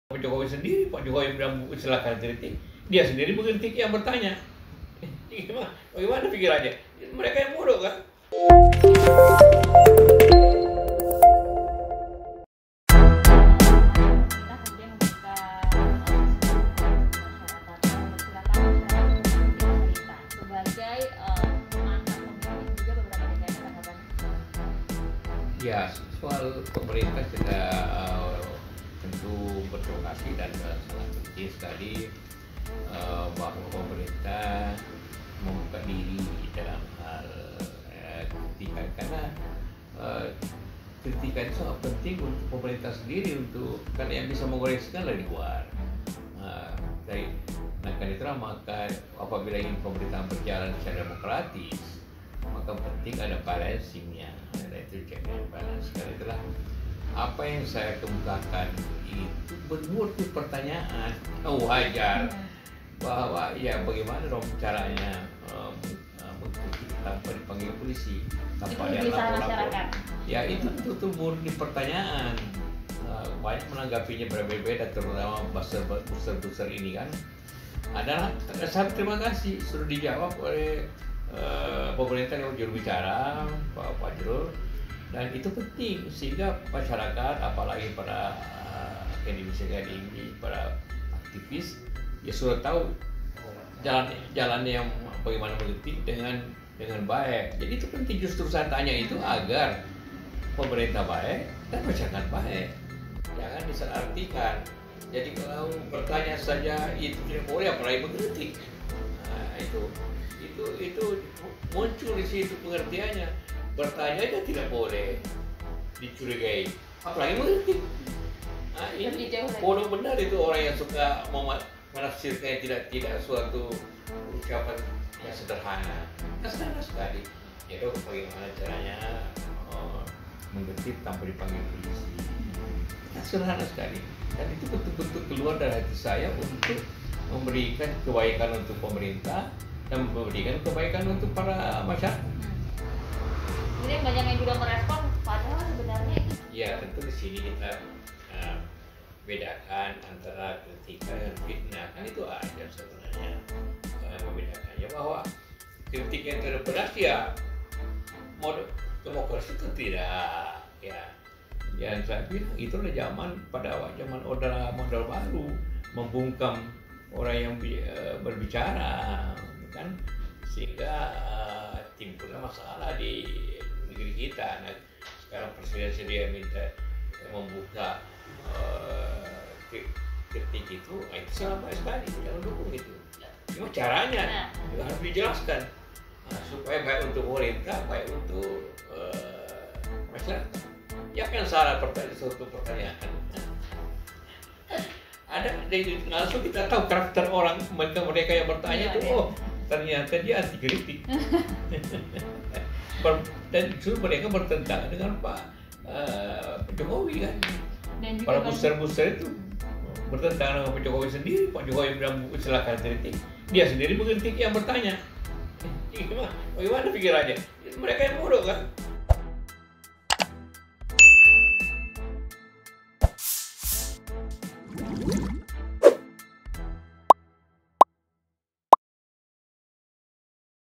Soal Kritik Tanpa Dipanggil Polisi, Ini Penjelasan LengKap JK
Mantan Wakil Presiden RI ke 10 dan 12, Jusuf Kalla (JK) meminta para pihak tidak salah mengartikan pertanyaannya terkait bagaimana cara mengkritik pemerintah tanpa dipanggil polisi.

Ia mengatakan hal itu mewakili kegelisahan masyarakat dan betujuan untuk kebaikan pemerintah itu sendiri.

Hal itu disampaikan JK di kediaman pribadinya Jl. Brawijaya No.6 Jakarta Selatan Kebayoran baru, senin 15 Februari 2021.

Video terkini dan terbaru..